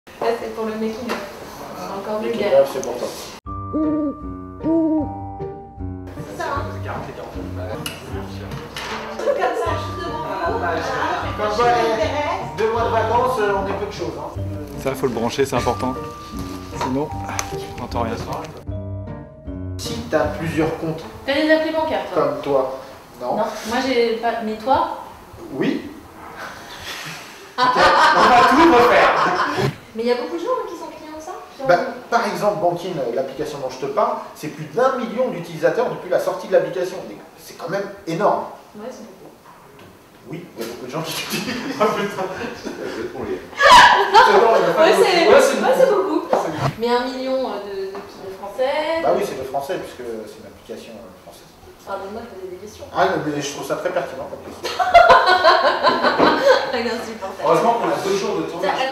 ONPDP - Dans les coulisses des Pigeons, pendant un tournage
Petit moment pendant le tournage de l'émission, avec Sébastien-Abdelhamid, et Claire Barsacq et tous ceux qui font l'émission, cadreurs, maquilleuse, ingénieurs du son...